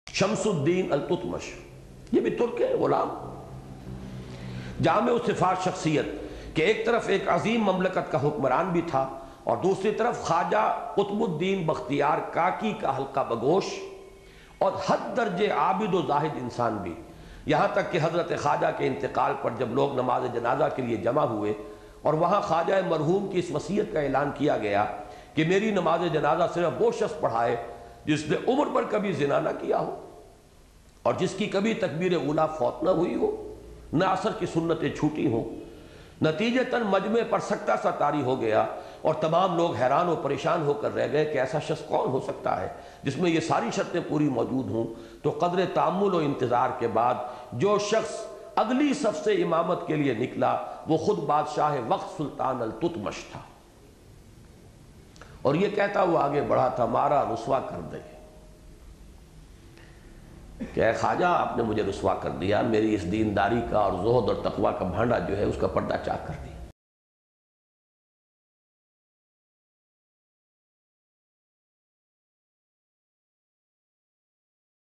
Dr israr Ahmad | Shams-ud-Din Al-Tutmash : Aik Derwaish Sift Hukmran
Lec #006
شمس الدین التتمش ایک درویش صفت حکمران